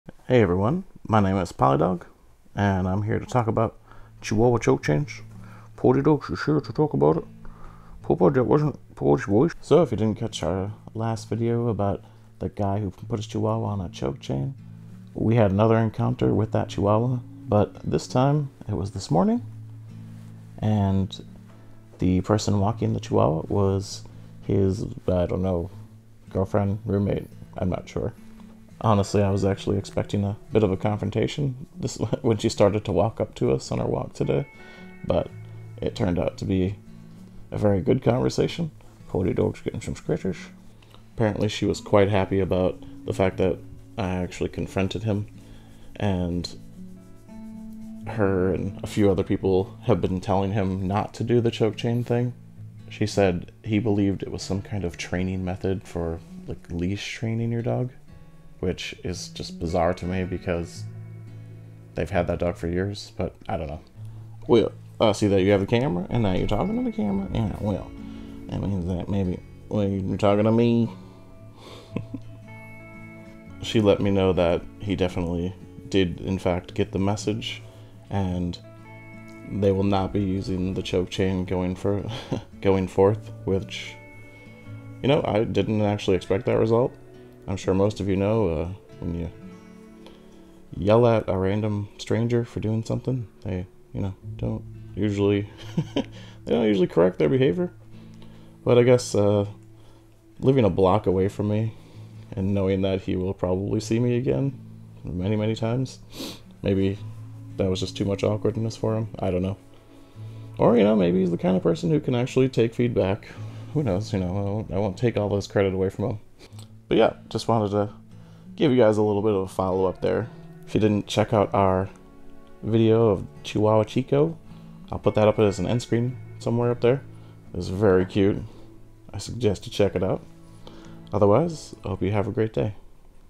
Second Encounter with the Chihuahua on a Choke Chain.
We ran in to the chihuahua who was on a choke chain this morning and we have some good news!

We have never asked for donations but for those who would like to do so.